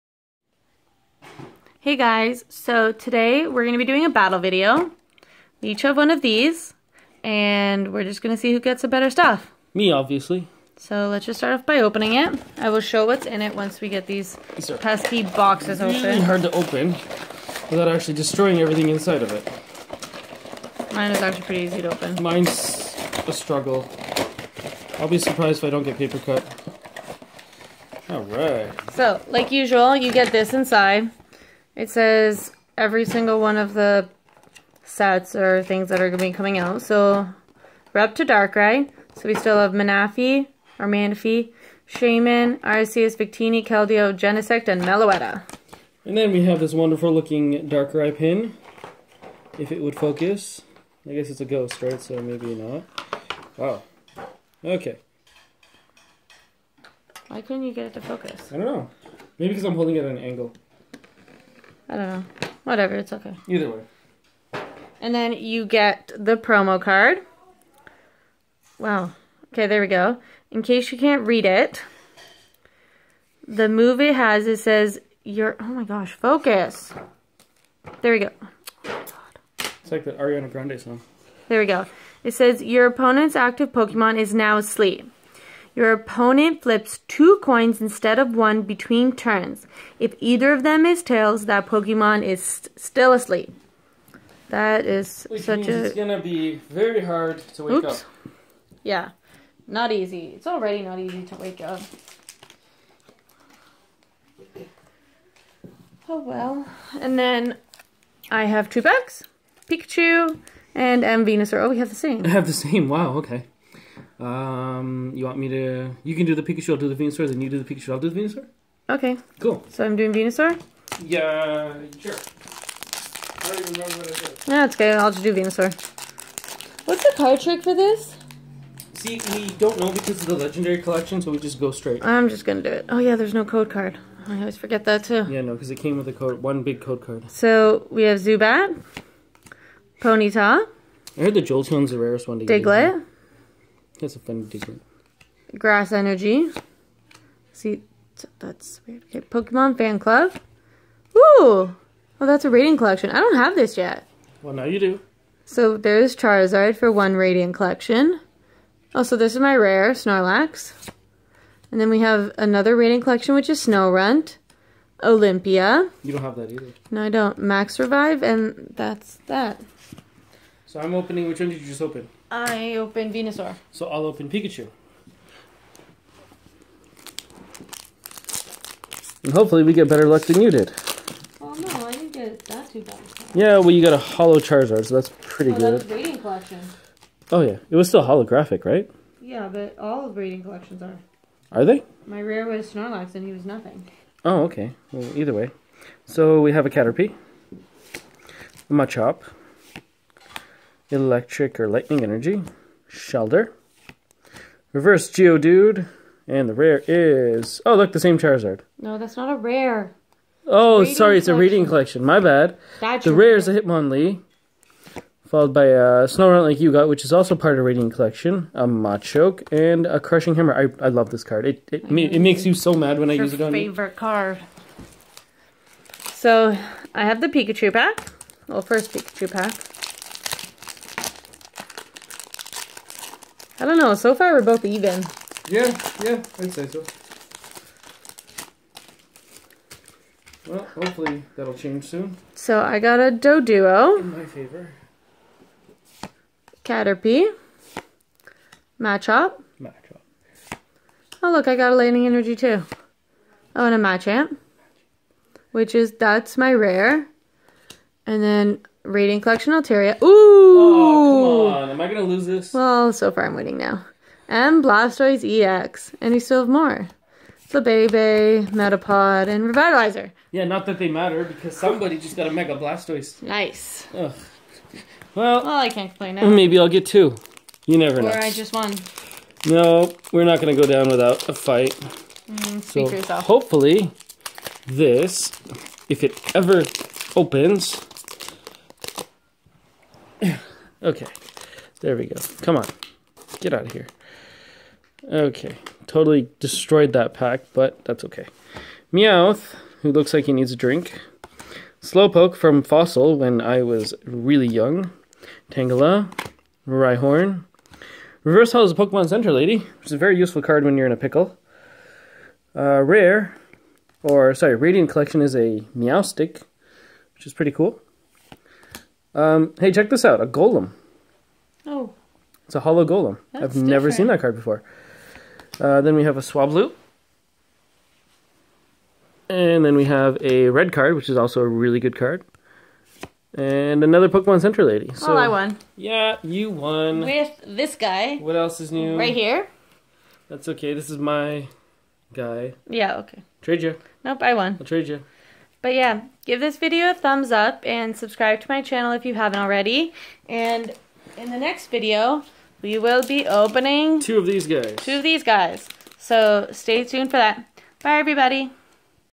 Pokemon - Darkrai Mythical Pokemon Collection Box Opening!
We open 2 boxes of the new Darkrai mythical pokemon collection to see who gets the better pulls.